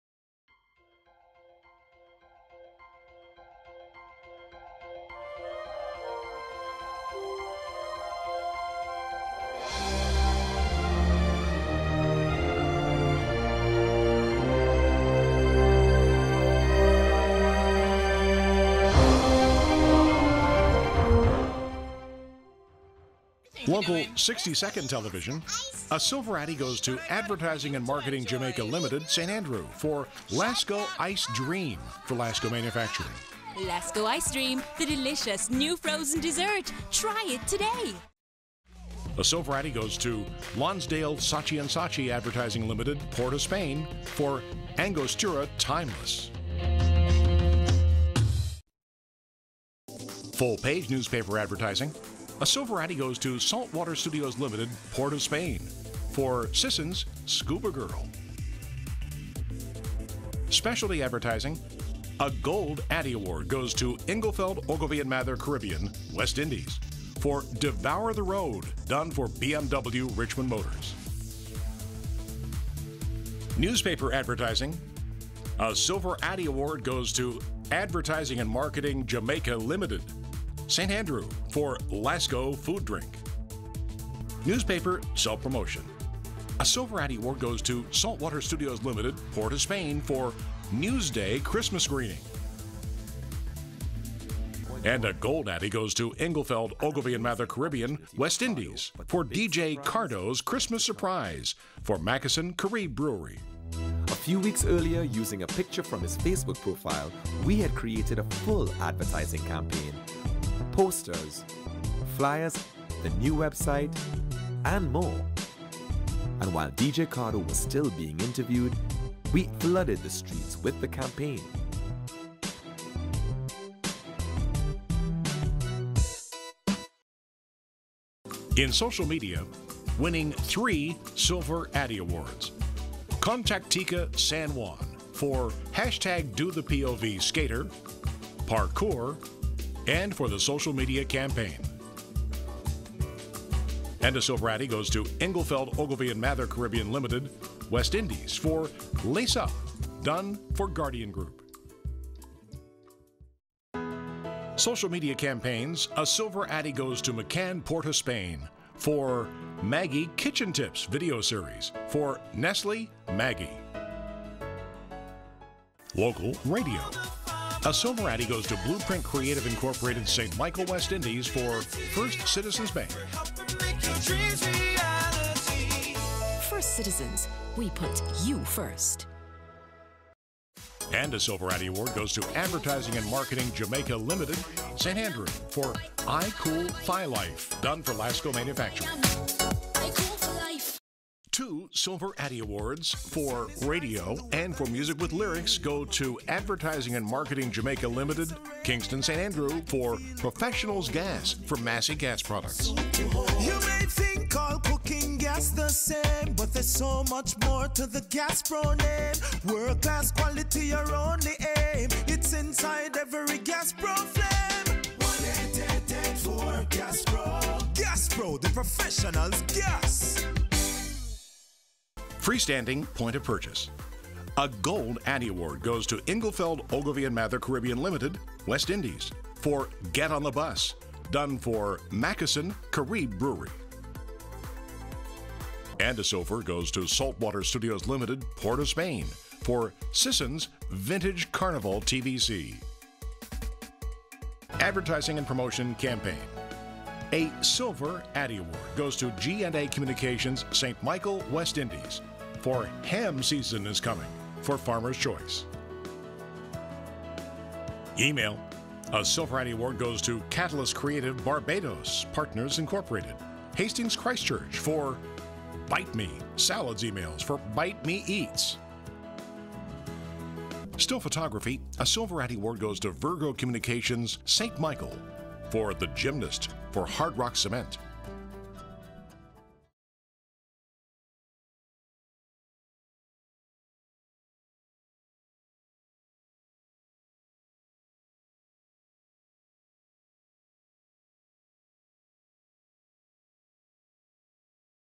Caf Addy District Winners 2017
The Florida district ADDY AWARDS 2017 in Miami.

Congratulations to all of the CAF "Caribbean winners"